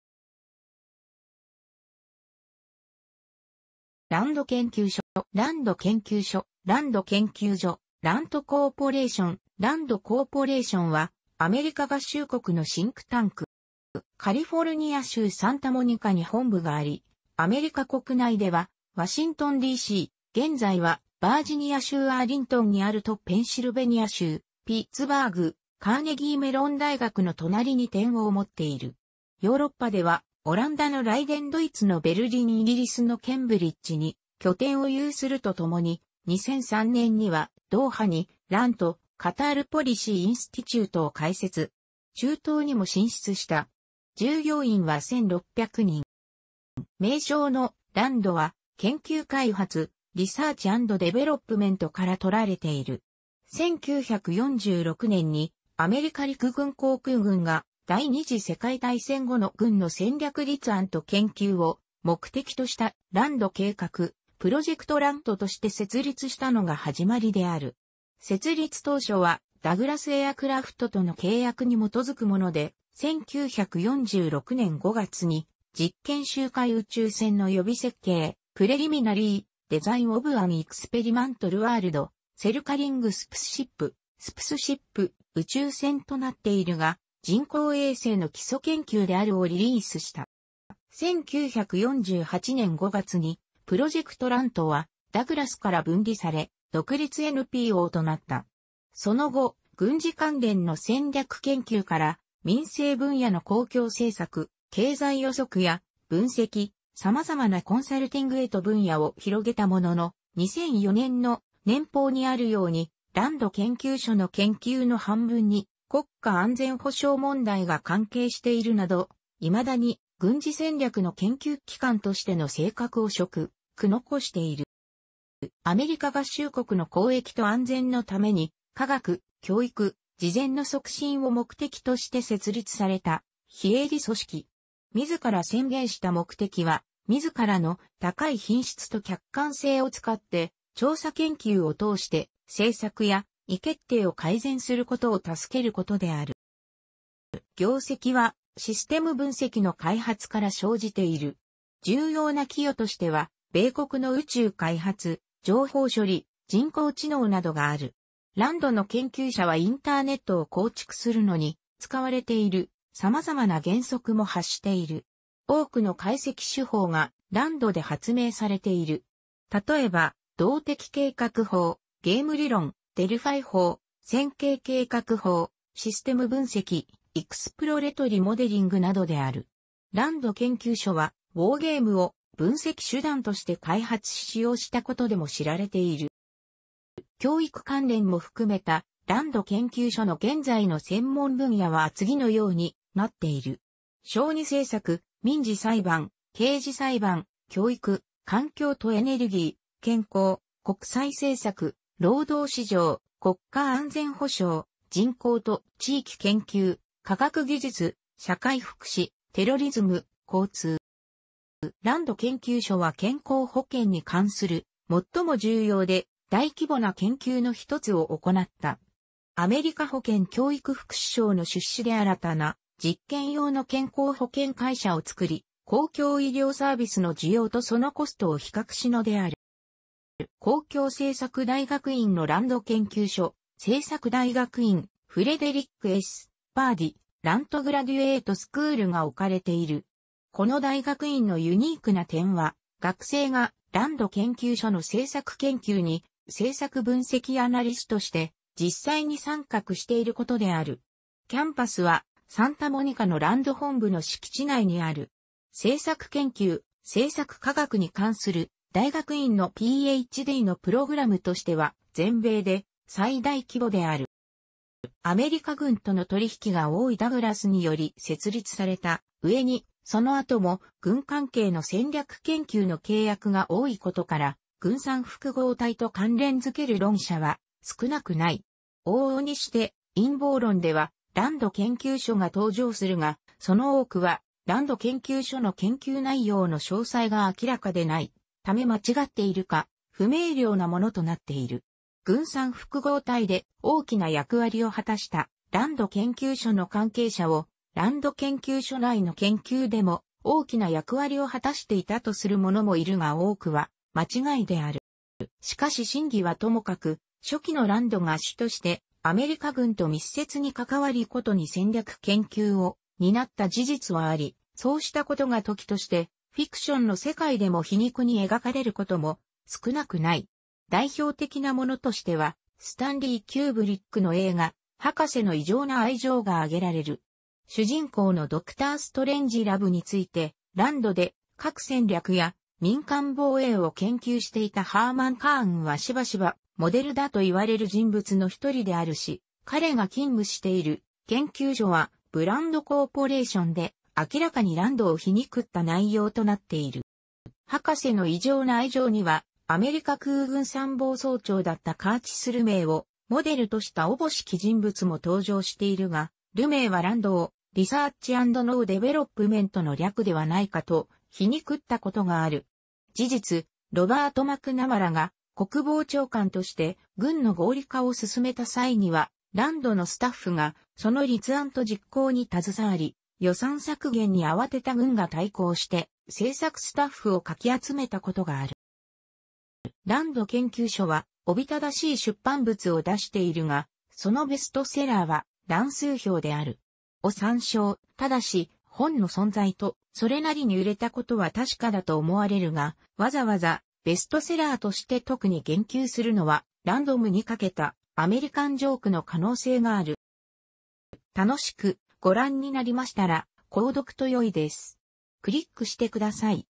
ランド研究所
ランド研究所（ランドけんきゅうじょ、RAND Corporation、ランド・コーポレーション）は、アメリカ合衆国のシンクタンク。

カリフォルニア州サンタモニカに本部があり、アメリカ国内ではワシントンD.C.（現在はヴァージニア州アーリントンにある）とペンシルベニア州ピッツバーグ（カーネギーメロン大学の隣）に拠点を持っている。ヨーロッパでは、オランダのライデン・ドイツのベルリン・イギリスのケンブリッジに拠点を有すると共に、2003年にはドーハに RAND-Qatar Policy Institute を開設。中東にも進出した。従業員は1600人。

名称の“ランド”は、研究開発（Research ANd Development）から取られている。
1946年にアメリカ陸軍航空軍が、第二次世界大戦後の軍の戦略立案と研究を目的とした 「ランド計画/Project RAND」として設立したのが始まりである。設立当初はダグラス・エアクラフトとの契約に基づくもので、1946年5月に「実験周回宇宙船の予備設計」（"Preliminary Design of an Experimental World-Circling Spaceship", Spaceship（宇宙船）となっているが、人工衛星の基礎研究である）をリリースした。

1948年5月に、「Project RAND」はダグラスから分離され、独立NPOとなった。その後、軍事関連の戦略研究から民生分野の公共政策・経済予測や分析、様々なコンサルティングへと分野を拡げたものの、2004年の年報にある様に「ランド研究所の研究の半分に国家安全保障問題が関係している」など、未だに軍事戦略の研究機関としての性格を色濃く残している。

「アメリカ合衆国の公益と安全のために、科学、教育、慈善の促進を目的として」設立された非営利組織。自ら宣言した目的は自らの「高い品質と客観性」を使って「調査研究を通して政策や意思決定を改善することを助ける」ことである。

業績はシステム分析の開発から生じている。重要な寄与としては、米国の宇宙開発、情報処理、人工知能などがある。ランドの研究者はインターネットを構築するのに使われている様々な原則も開発している。多くの解析手法がランドで発明されている。例えば、動的計画法、ゲーム理論、デルファイ法、線形計画法、システム分析、exploratory modeling などである。ランド研究所はウォーゲームを分析手段として開発し使用したことでも知られている。

教育関連も含めたランド研究所の現在の専門分野は次のようになっている。小児政策、民事裁判、刑事裁判、教育、環境とエネルギー、健康、国際政策、労働市場、国家安全保障、人口と地域研究、科学技術、社会福祉、テロリズム、交通。

ランド研究所は健康保険に関する最も重要で大規模な研究のひとつを行った。アメリカ保健教育福祉省の出資で新たな実験用の健康保険会社を作り、公共医療サービスの需要とそのコストを比較したのである。

公共政策大学院のランド研究所政策大学院（Frederick S. Pardee RAND Graduate School）が置かれている。この大学院のユニークな点は学生がランド研究所の政策研究に政策分析アナリストとして実際に参画していることである。キャンパスはサンタモニカのランド本部の敷地内にある。政策研究、政策科学に関する大学院のPh.D.のプログラムとしては全米で最大規模である。

アメリカ軍との取引が多いダグラスにより設立された上に、その後も軍関係の戦略研究の契約が多いことから、軍産複合体と関連付ける論者は少なくない。往々にして陰謀論ではランド研究所が登場するが、その多くはランド研究所の研究内容の詳細が明らかでないため間違っているか不明瞭なものとなっている。軍産複合体で大きな役割を果たしたランド研究所の関係者を、ランド研究所内の研究でも大きな役割を果たしていたとする者もいるが多くは間違いである。

しかし真偽は兎も角、初期のランドが主としてアメリカ軍と密接に関わり殊に戦略研究を担った事実はあり、そうしたことが時としてフィクションの世界でも皮肉に描かれることも少なくない。代表的なものとしては、スタンリー・キューブリックの映画『博士の異常な愛情』が挙げられる。主人公のドクター・ストレンジラブについて、ランドで核戦略や民間防衛を研究して...